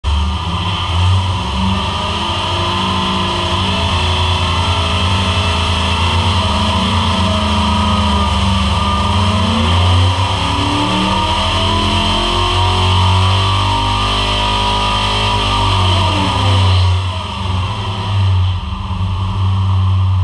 427 SBC
We built and tested a 427 SBC. We used customer furnished AFR heads. We started with a Dart block and added a custom rotating assembly. Comp Cams ground us a small base circle Hydraulic roller. The engine is going into a  '91 Corvette with factory fuel injection that the customer is going to tune. We tested it with our high rise single plane intake. A 780 QuickFuel and GM HEI distributor. The motor was pretty square. It did 523 HP and 500 lbs-ft of torque.